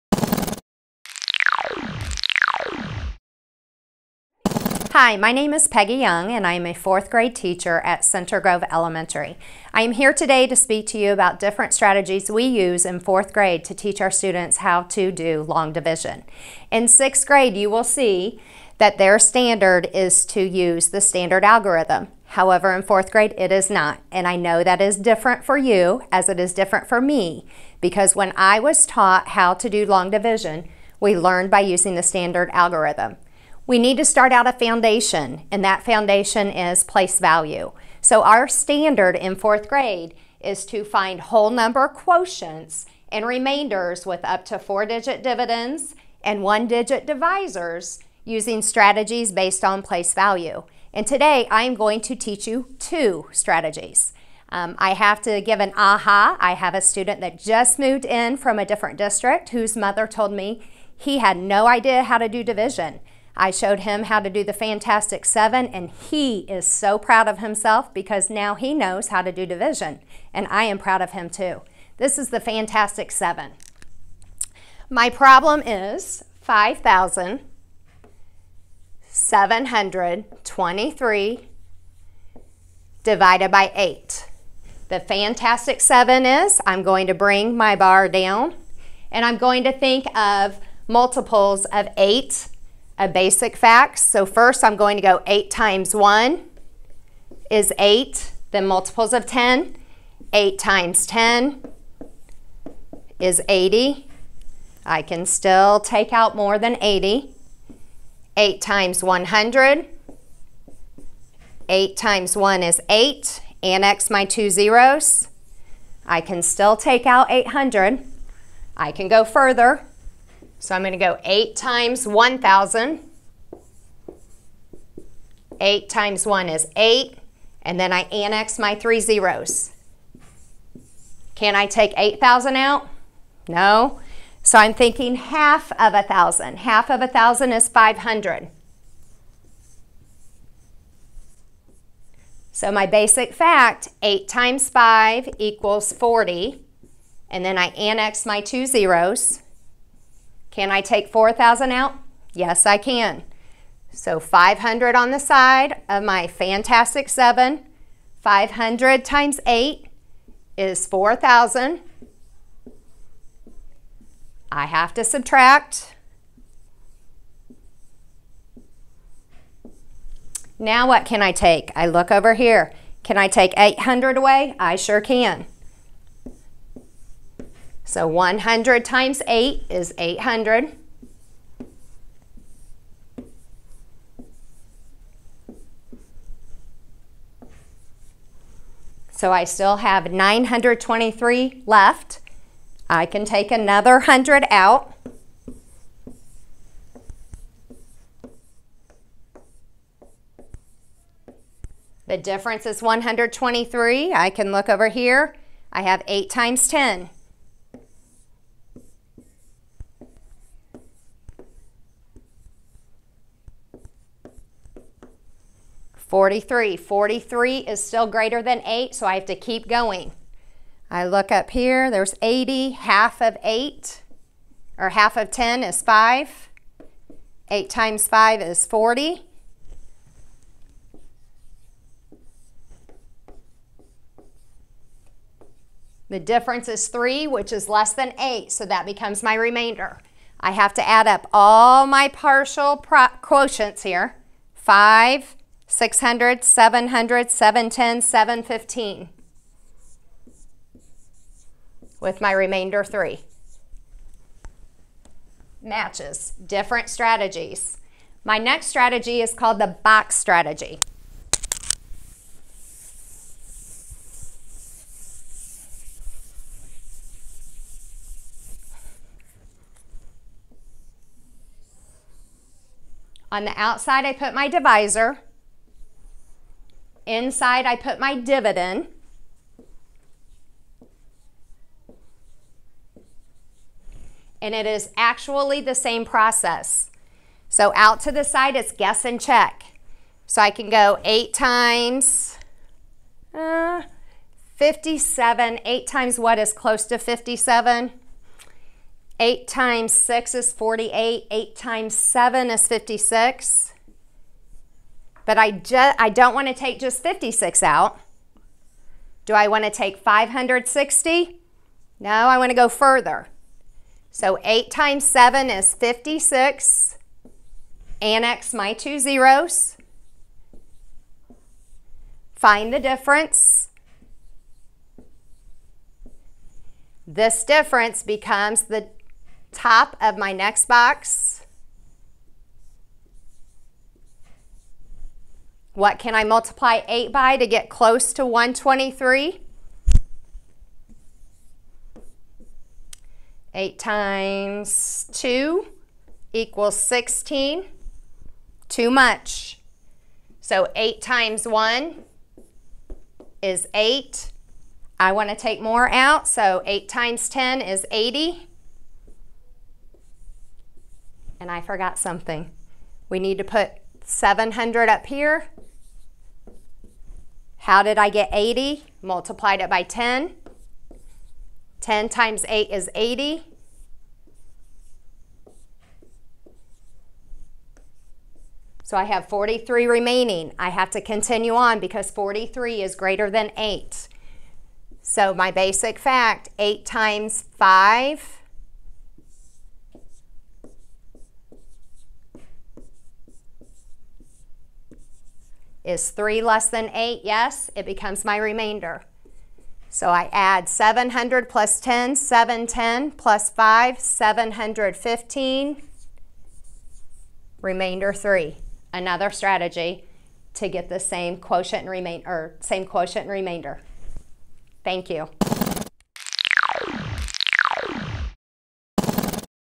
Learn CG: Grade 4 Long Division Strategies
Center Grove Elementary 4th Grade teacher Peggy Young shares long division strategies.